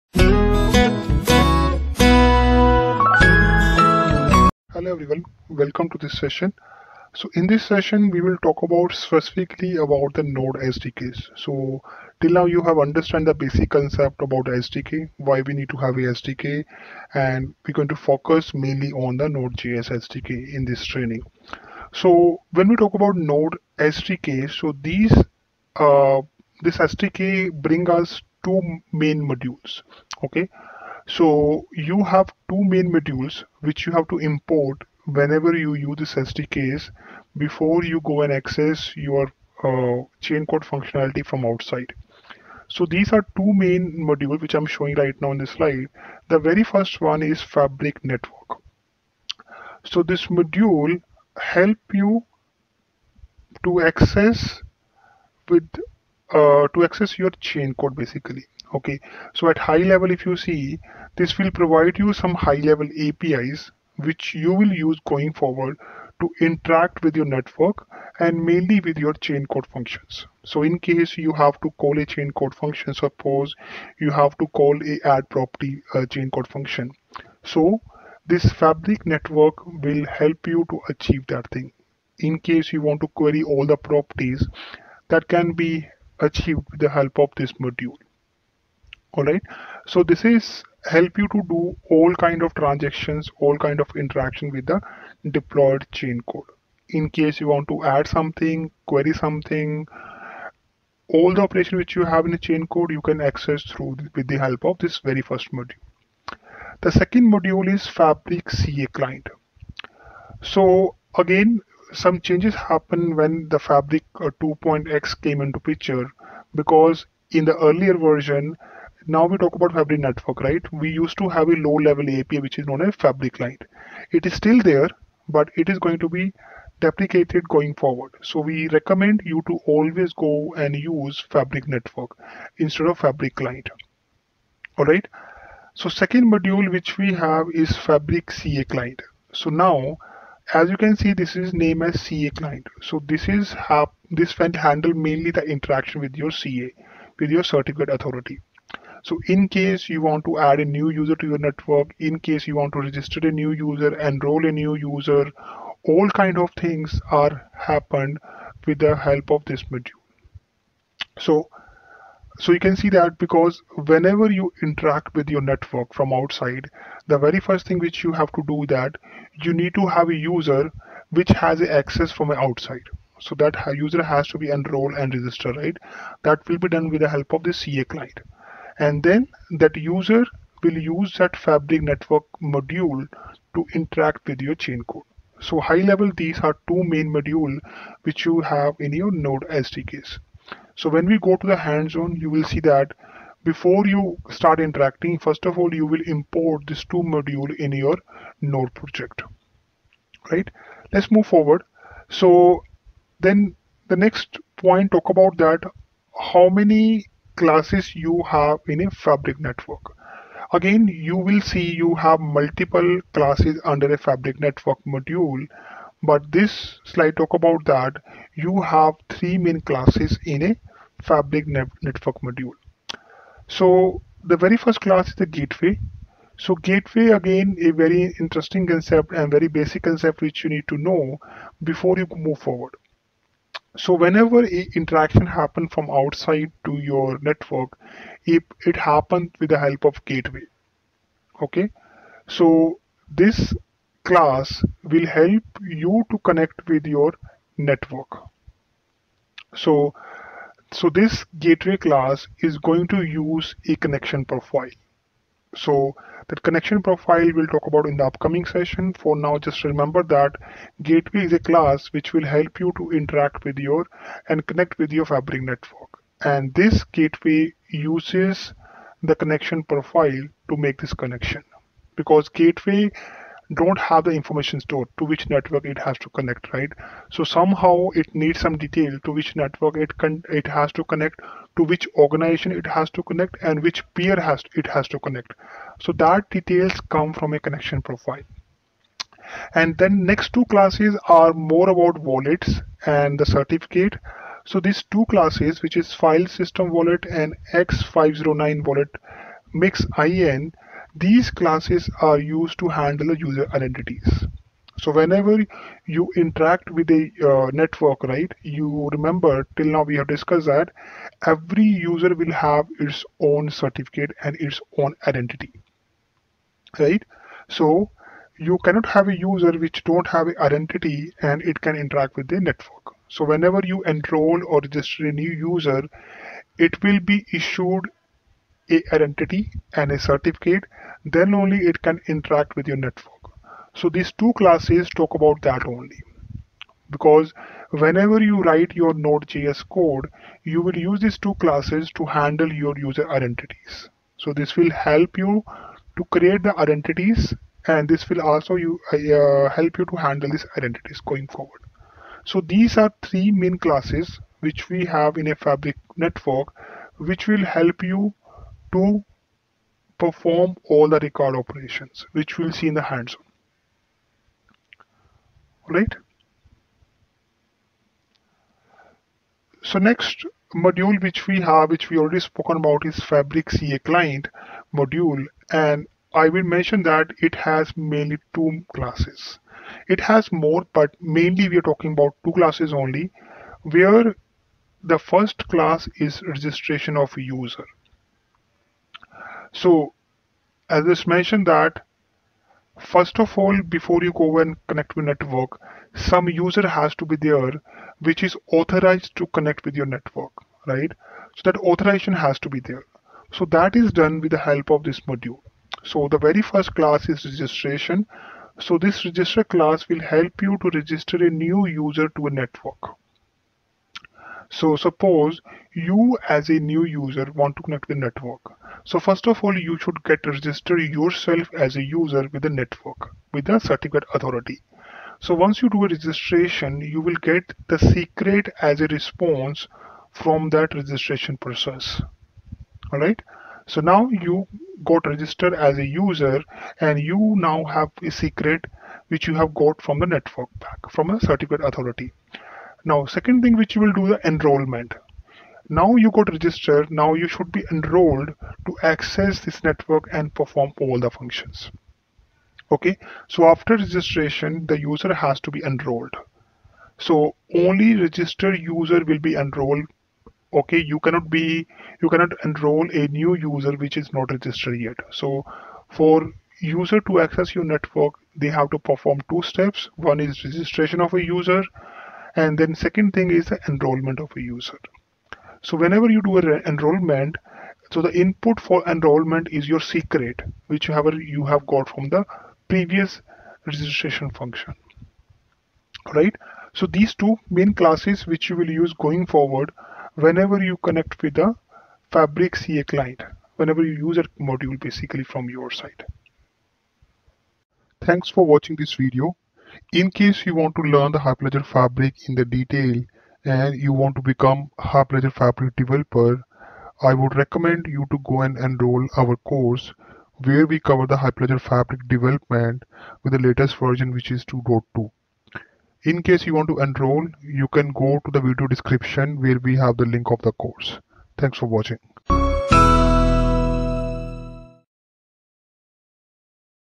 Blockchain Tutorials | Hyperledger Fabric 2.2 Node SDK Modules
In this video, you will learn and understand about Hyperledger Fabric Node SDK Modules.

Enroll to our course to learn more about Hyperledger fabric development.